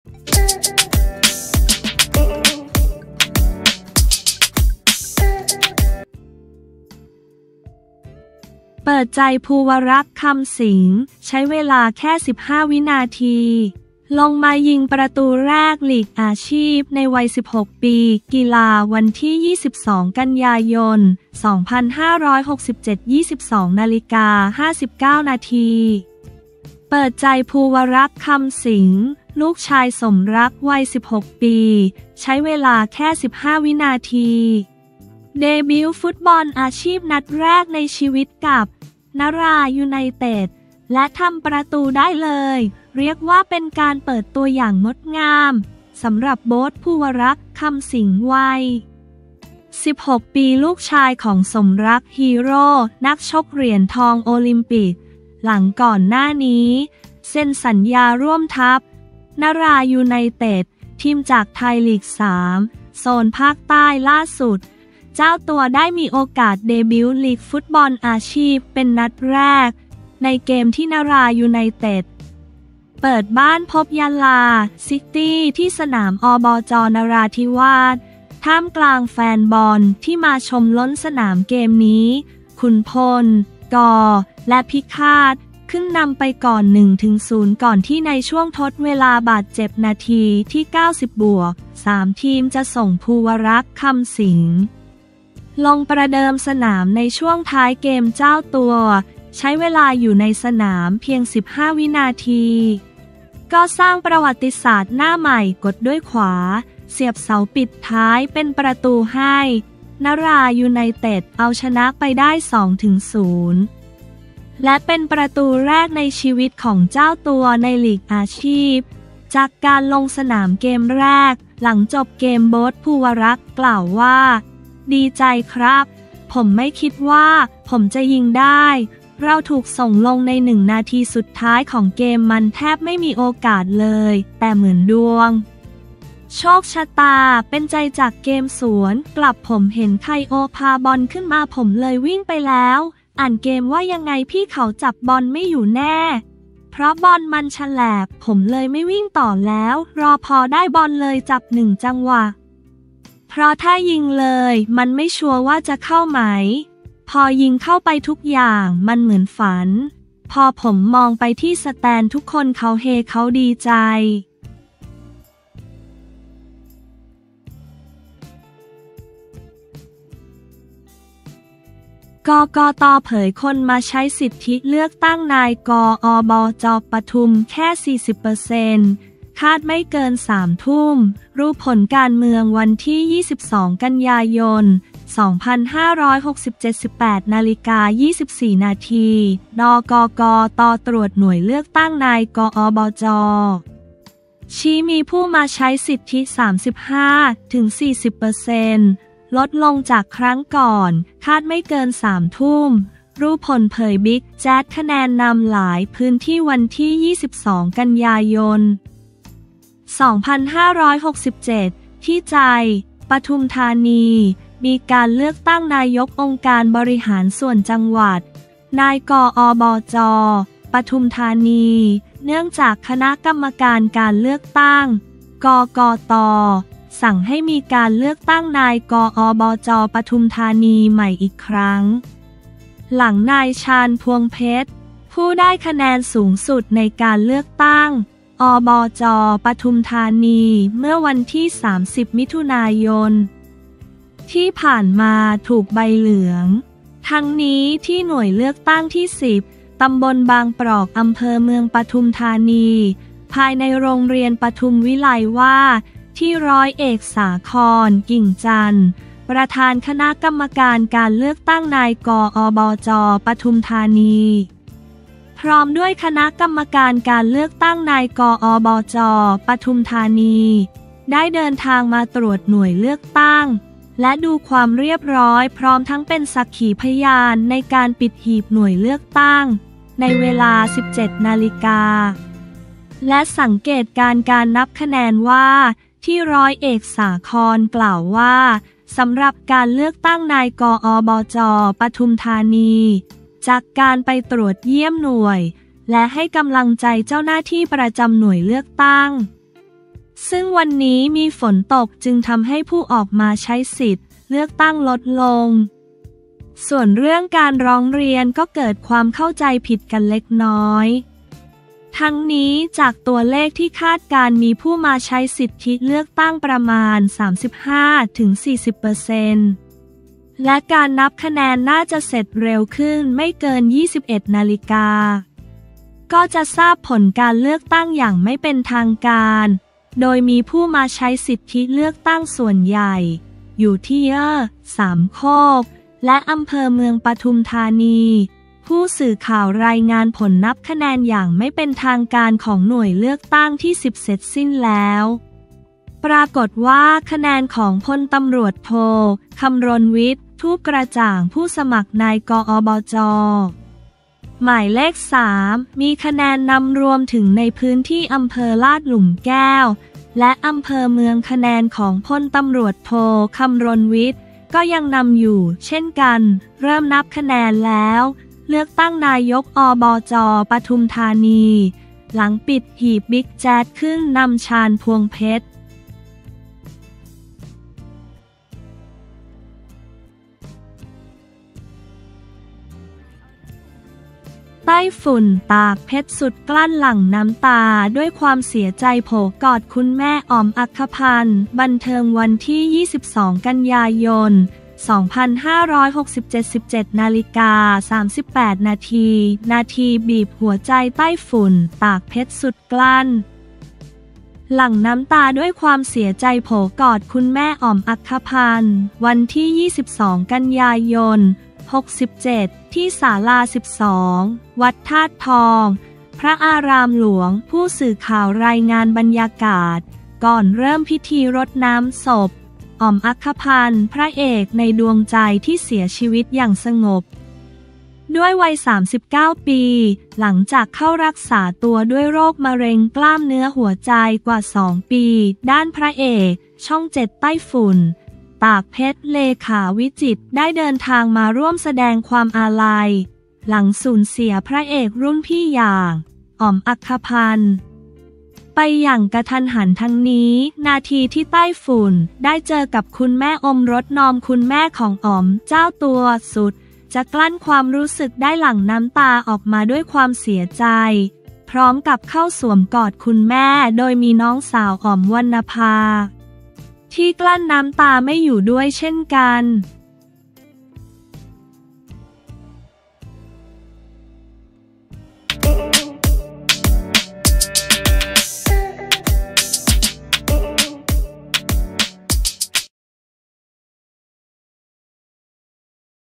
เปิดใจ ภูวรักษ์ คำสิงห์ ใช้เวลาแค่ 15 วินาที ลงมายิงประตูแรกลีกอาชีพในวัย 16 ปี
กีฬา
22 ก.ย. 2567
22:59 น.
เปิดใจ ภูวรักษ์ คำสิงห์ ลูกชาย สมรักษ์ วัย 16 ปี ใช้เวลาแค่ 15 วินาที เดบิวต์ฟุตบอลอาชีพนัดแรกในชีวิตกับ นรา ยูไนเต็ด และทำประตูได้เลย

เรียกว่าเป็นการเปิดตัวอย่างงดงาม สำหรับ โบ๊ท-ภูวรักษ์ คำสิงห์ วัย 16 ปี ลูกชายของ สมรักษ์ ฮีโร่นักชกเหรียญทองโอลิมปิก หลังก่อนหน้านี้เซ็นสัญญาร่วมทัพ นรา ยูไนเต็ด ทีมจากไทยลีก 3 โซนภาคใต้

ล่าสุด เจ้าตัวได้มีโอกาสเดบิวต์ลีกฟุตบอลอาชีพเป็นนัดแรก ในเกมที่ นรา ยูไนเต็ด เปิดบ้านพบ ยะลา ซิตี้ ที่สนาม อบจ.นราธิวาส ท่ามกลางแฟนบอลที่มาชมล้นสนาม
เกมนี้ขุนพล “กอและพิฆาต” ขึ้นนำไปก่อน 1-0 ก่อนที่ในช่วงทดเวลาบาดเจ็บนาทีที่ 90+3 ทีมจะส่ง ภูวรักษ์ คำสิงห์ ลงประเดิมสนามในช่วงท้ายเกม

เจ้าตัวใช้เวลาอยู่ในสนามเพียง 15 วินาที ก็สร้างประวัติศาสตร์หน้าใหม่ กดด้วยขวาเสียบเสาปิดท้ายเป็นประตูให้ นรา ยูไนเต็ด เอาชนะไปได้ 2-0 และเป็นประตูแรกในชีวิตของเจ้าตัวในลีกอาชีพ จากการลงสนามเกมแรก

หลังจบเกม โบ๊ท-ภูวรักษ์ กล่าวว่า “ดีใจครับ ผมไม่คิดว่าผมจะยิงได้ เราถูกส่งลงใน 1 นาทีสุดท้ายของเกม มันแทบไม่มีโอกาสเลย แต่เหมือนดวง โชคชะตาเป็นใจ จากเกมสวนกลับผมเห็นไคโอ พาบอลขึ้นมา ผมเลยวิ่งไป แล้วอ่านเกมว่ายังไงพี่เขาจับบอลไม่อยู่แน่ เพราะบอลมันแฉลบ ผมเลยไม่วิ่งต่อแล้วรอ พอได้บอลเลยจับ 1 จังหวะ เพราะถ้ายิงเลยมันไม่ชัวร์ว่าจะเข้าไหม พอยิงเข้าไปทุกอย่างมันเหมือนฝัน พอผมมองไปที่สแตนด์ ทุกคนเขาเฮ เขาดีใจ